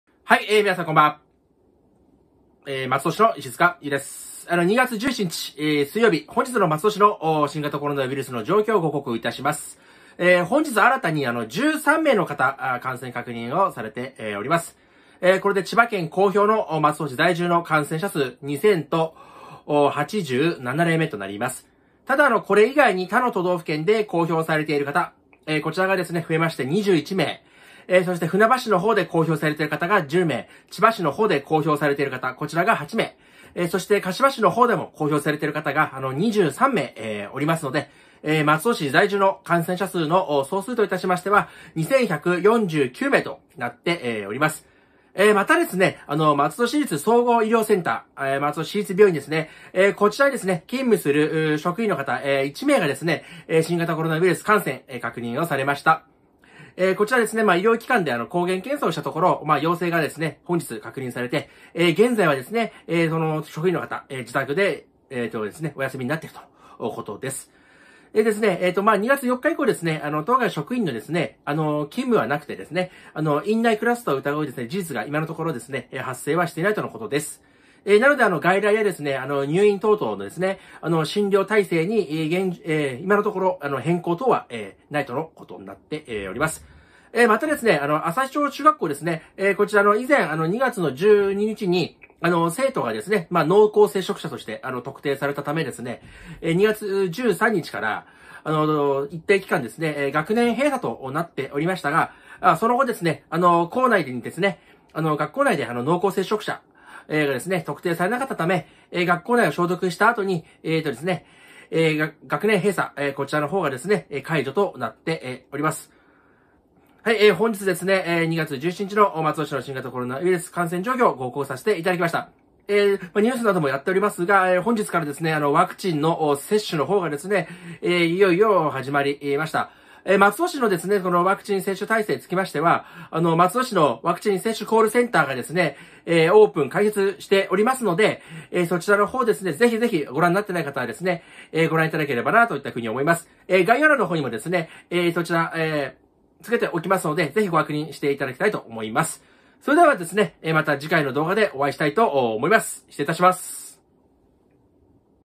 【松戸市議会・石塚ゆう】ワクチン接種開始・松戸市立総合医療センター職員の新型コロナウイルス感染が確認・旭町中学校の学年閉鎖解除・2/17（水）松戸市の新型コロナウイルス感染状況
こんにちは！松戸市議会に挑戦中の石塚ゆうです。

2月17日（水）松戸市の新型コロナウイルス感染状況、ワクチン接種開始、松戸市立総合医療センター職員の新型コロナウイルス感染について。

松戸市の感染状況詳細は松戸市HPをご確認ください。

■松戸市HP《松戸市立総合医療センター》

■松戸市ワクチン接種コールセンター・厚生労働省ワクチン接種コールセンター

石塚ゆう公式ブログ